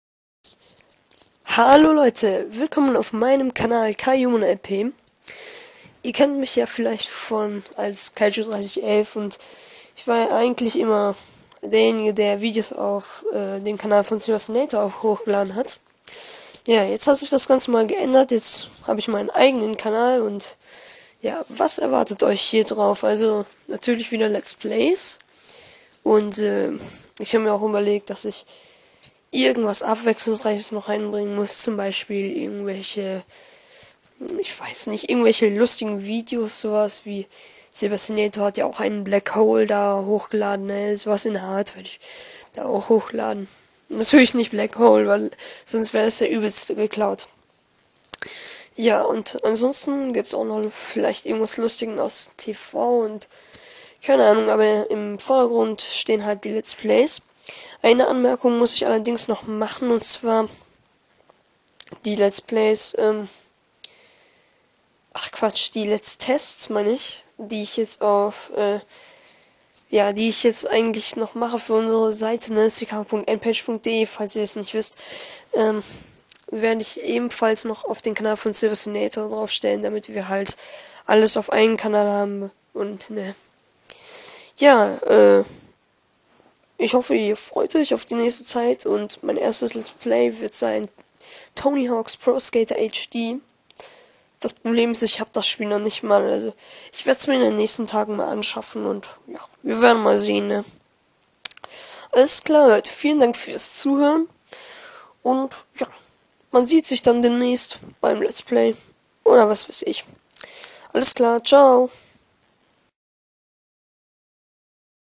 (Info) Willkommen auf meinem Kanal
In diesem Video stelle ich euch meinen Kanal vor und was ich in Zukunft so alles macen werde. Viel Spaß!